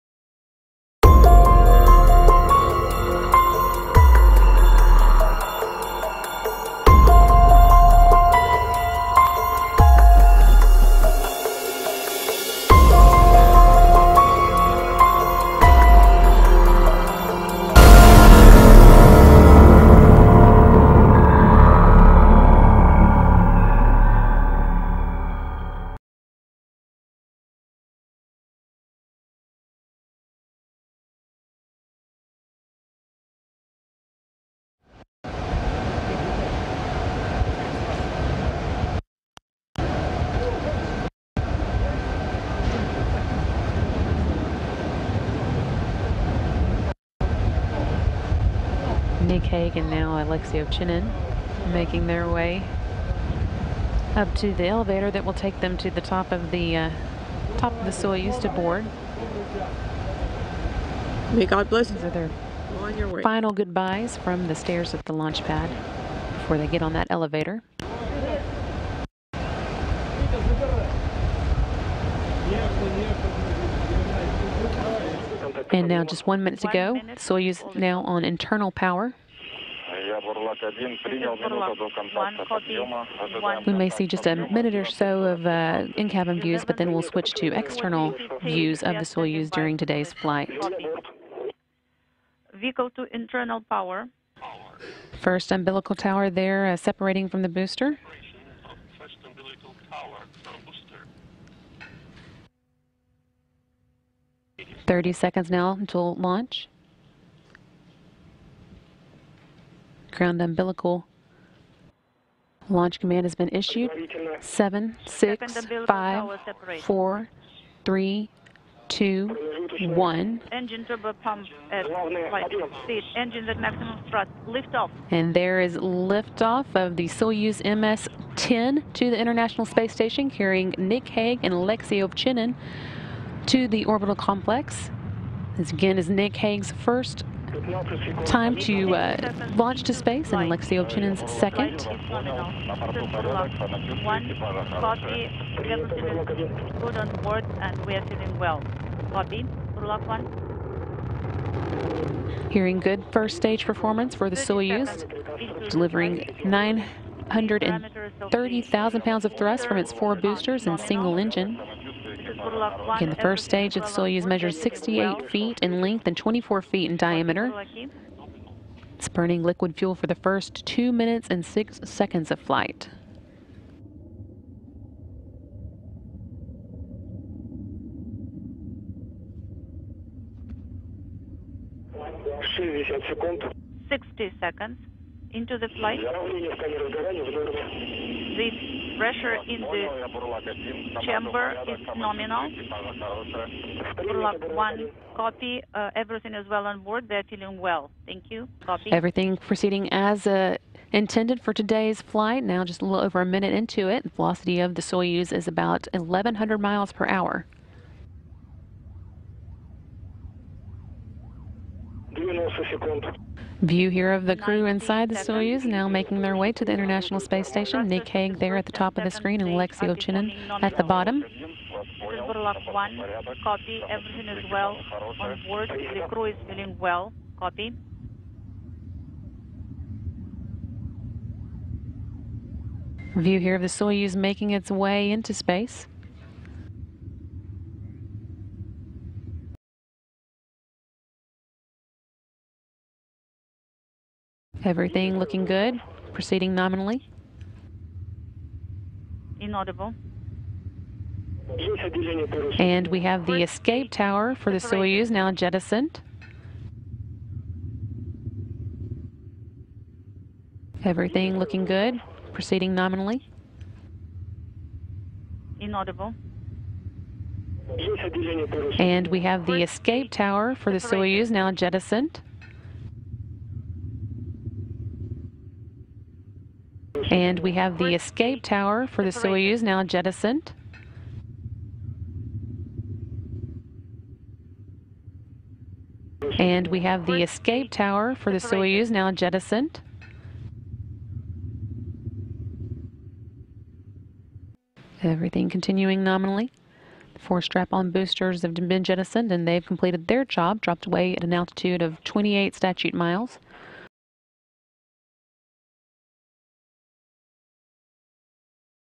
Last few seconds of Astronauts in Soyuz MS10 rocket Failure Episode 62
11 October 2018 || Breaking NEWS!!
There had been a booster failure aboard the Soyuz MS-10 mission to the ISS. The crew had aborted the mission to the ISS and had an emergency descent. They all are said to be safe.